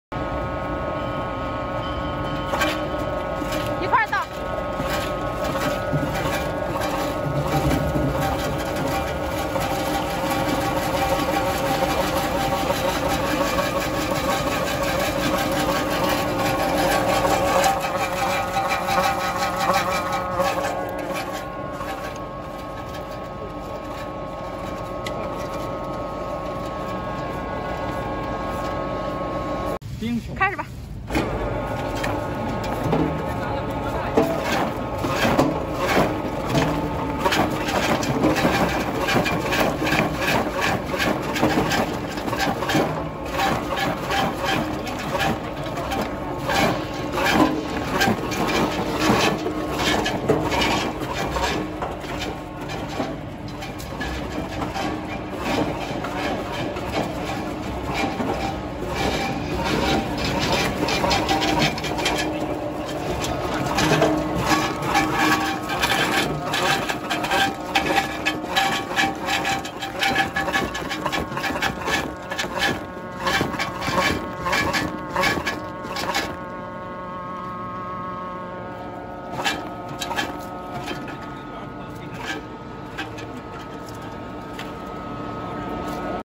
6 MM Onion Carrot Dicer Chopper Machine Commercial
This onion and carrot dicing machine can diced radish, potatoes, sweet potatoes, taro, bamboo shoots, onions, eggplants, apples, ginger and many other melons.
Powerful function, high output, easy to replace the cutter head, easy to operate, easy to clean, this bulb vegetable cutting machine is suitable for food processing plants, catering industries, canteens, and catering distribution centers.